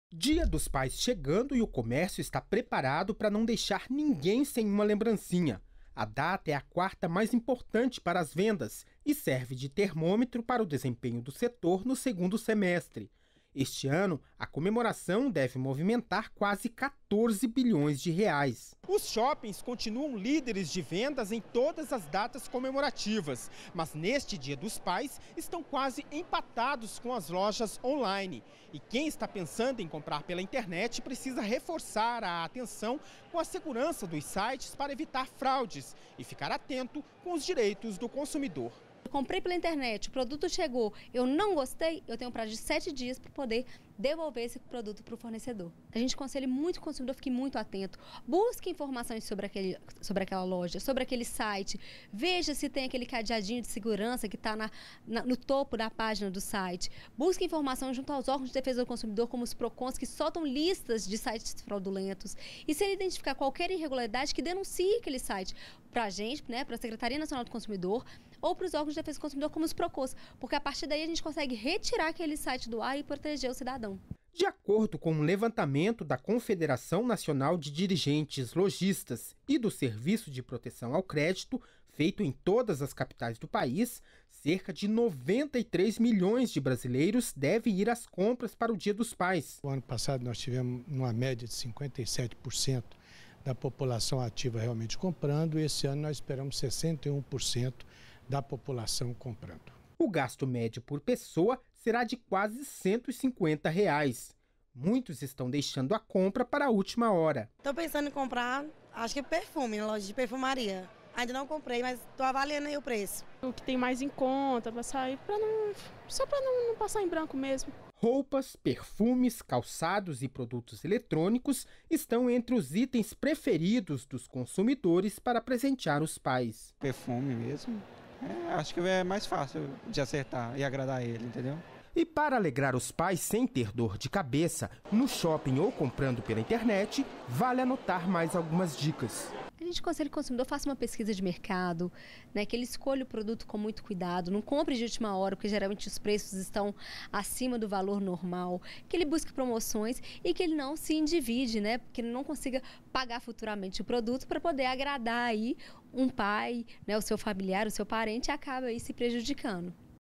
Dia dos pais traz boas expectativas para as vendas das lojas pela internet
REPÓRTER NBR - 09.08.18: no próximo domingo (12) é dia dos pais, e a expectativa  do comércio para este ano é que as vendas das lojas pela internet se aproximem dos shoppings, que são os líderes nas comemorações.